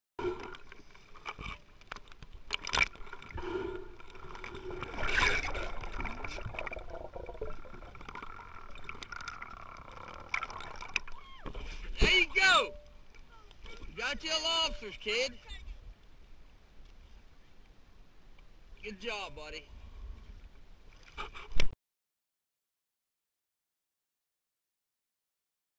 DYLAN NABBING UP SOME FLORIDA KEYS LOBSTER
DYLAN AT 11 YEARS OLD CATCHING LOBSTER